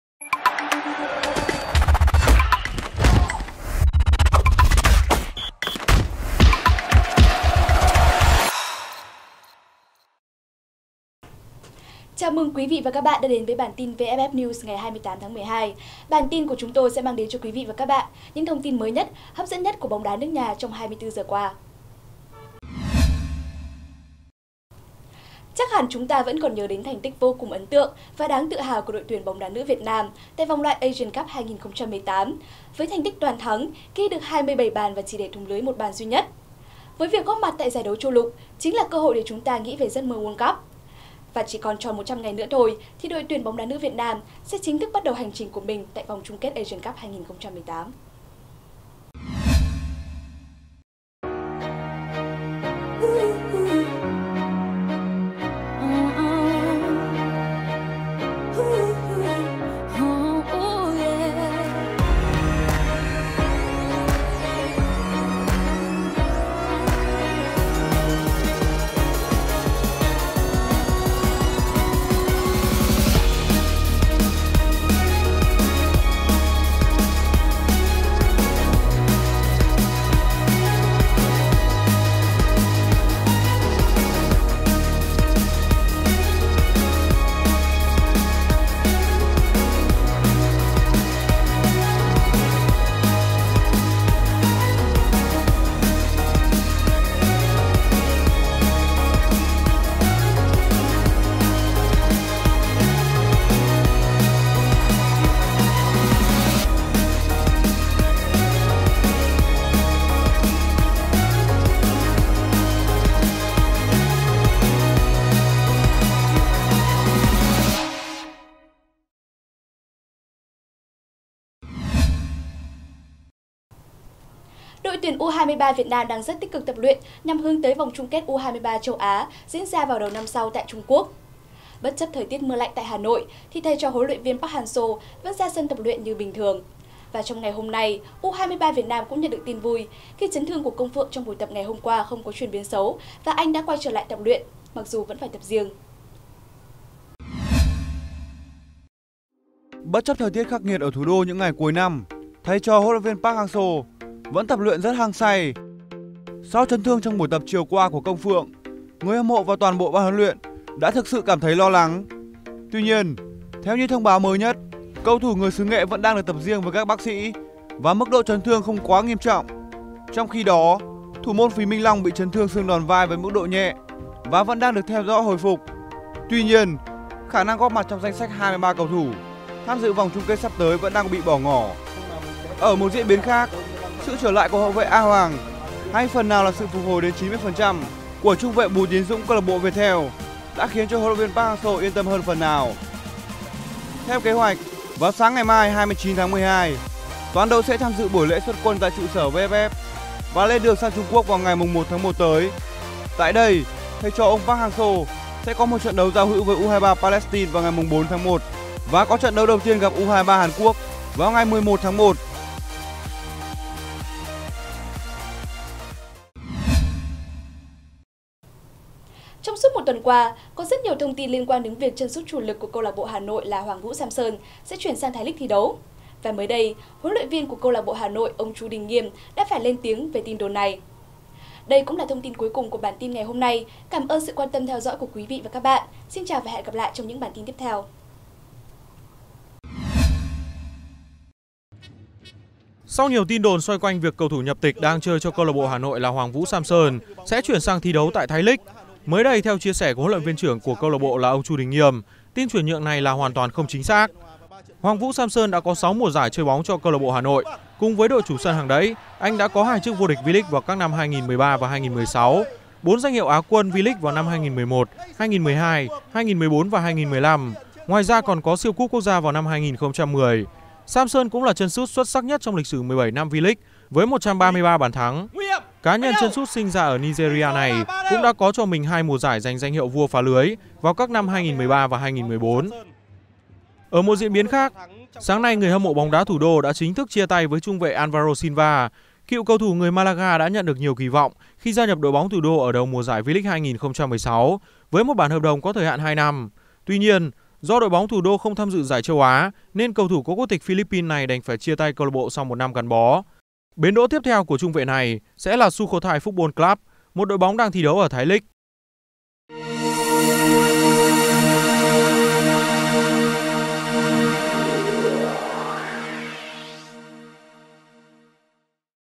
VFF NEWS SỐ 84 | Công Phượng chỉ gặp phải chấn thương nhẹ và vẫn sẽ tham dự VCK U23 châu Á
VFF Channel | VFF NEWS SỐ 84 | Công Phượng chỉ gặp phải chấn thương nhẹ và vẫn sẽ tham dự VCK U23 châu Á